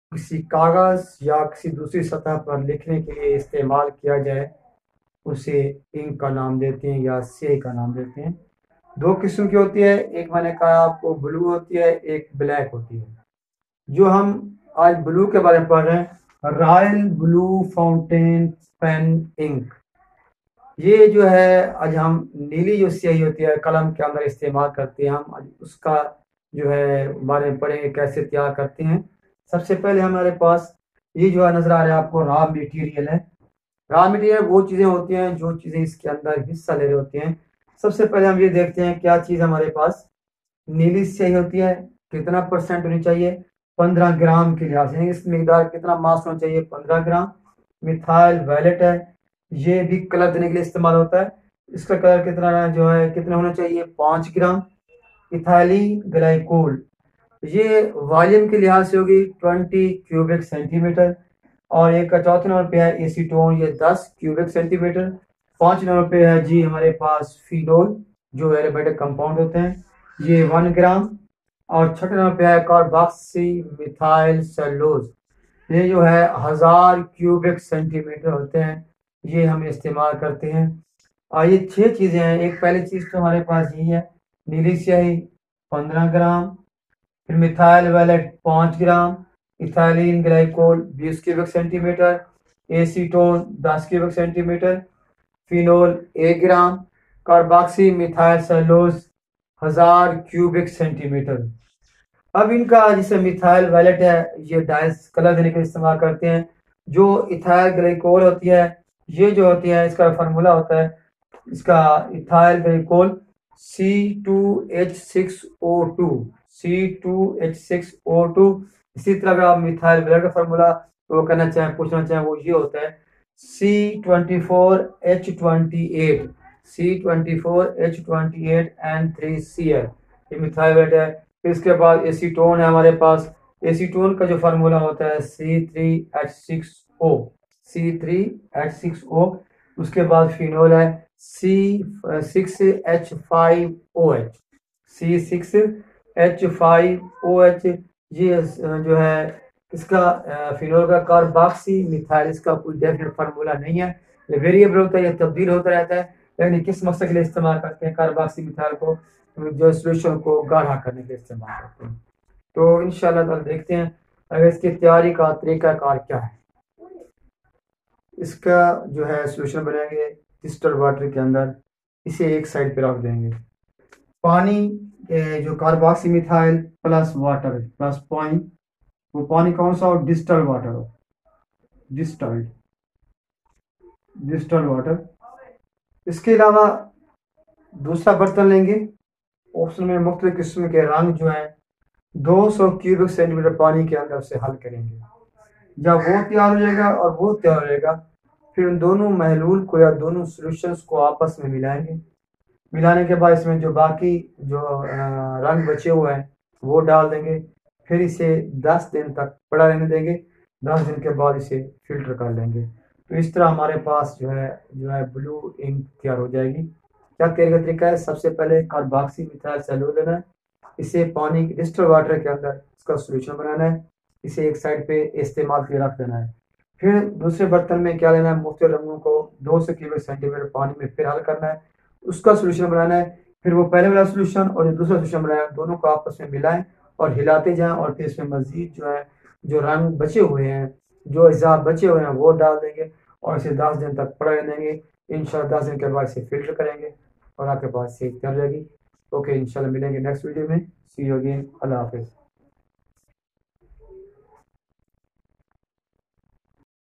Royel Blue Fountain Ink Pen in Chemistry and its Preparation.
In this video you will learn about blue fountain ink pen and its preparation You will also learn about the ink definition.And its components which are used in making blue fountain ink pen.